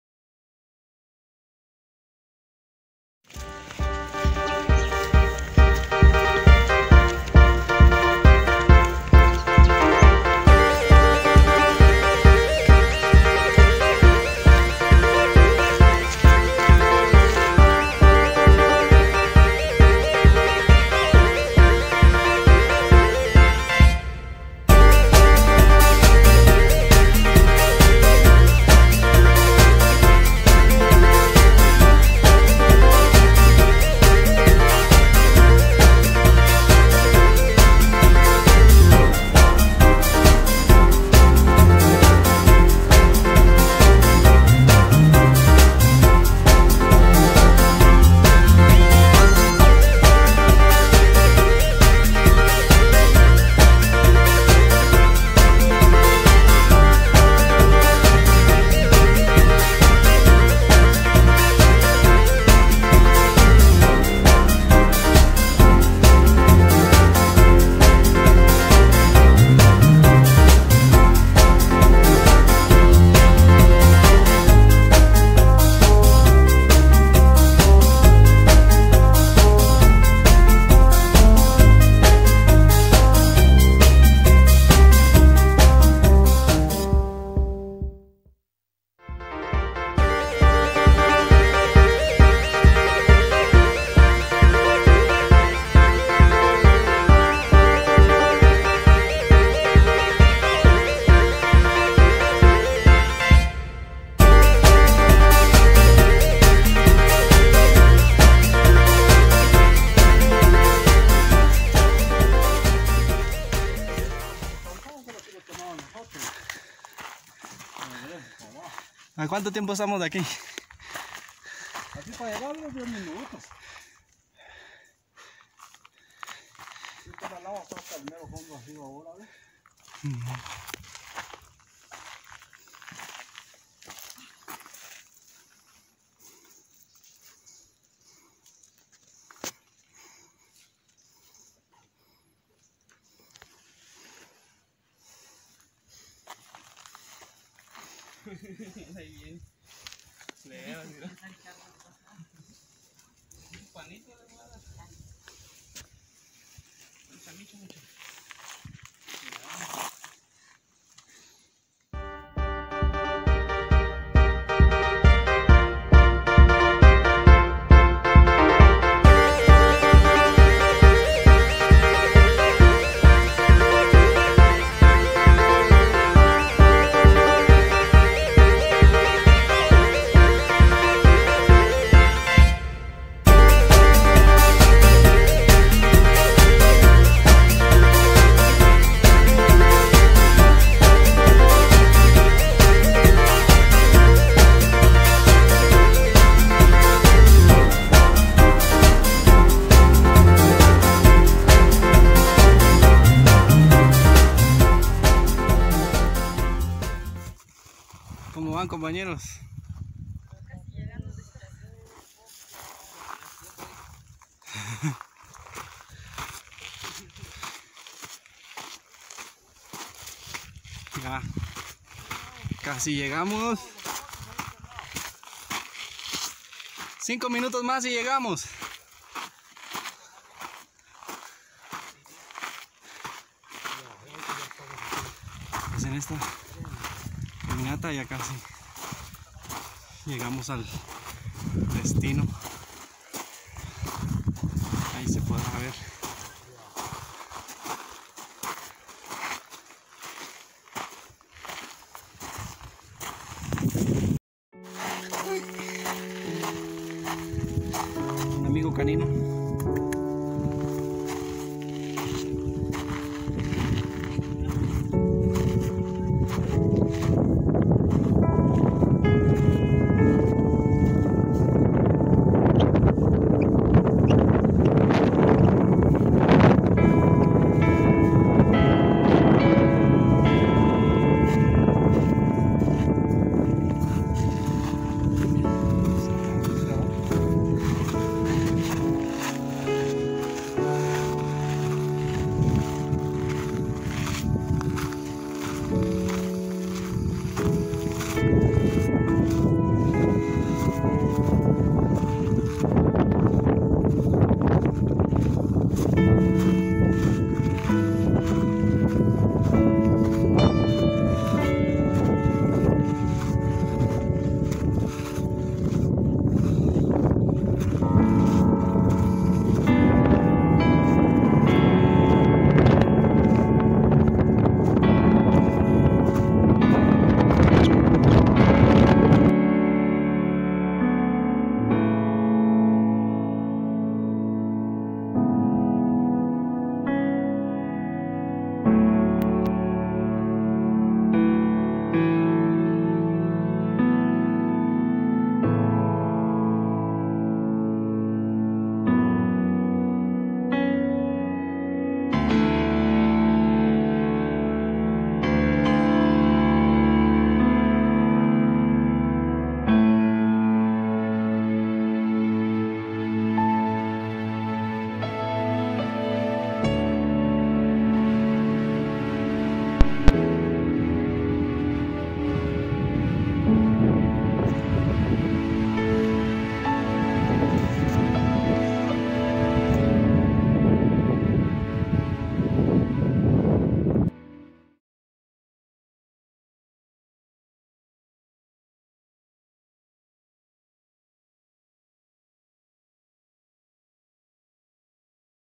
Rumbo a Volcan de Pacaya
Subiendo al Volcan de Pacaya, San Vicente Pacaya, Escuintla.